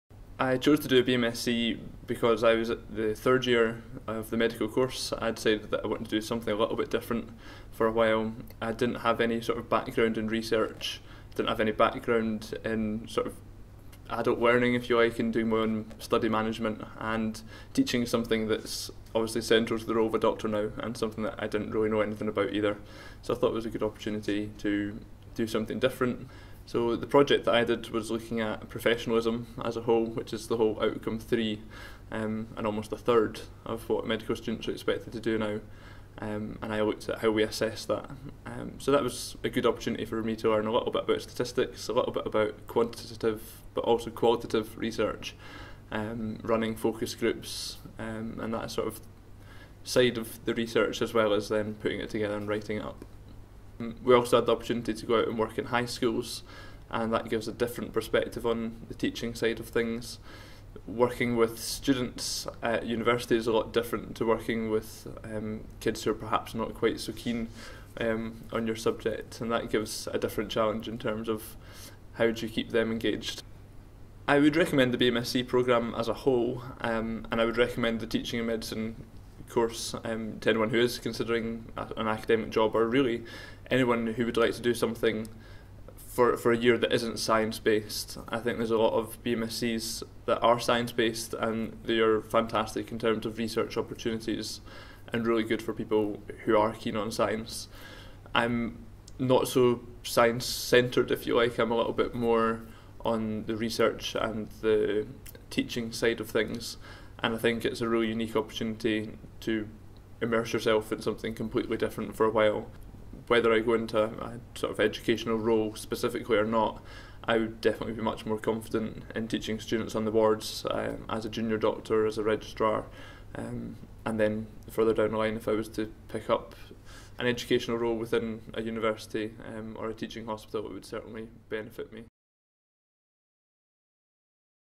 BMSc Medical Education at the University of Dundee
Zach Thomson speaks about his experience studying Medical Education as an intercalated BMSc option at the University of Dundee.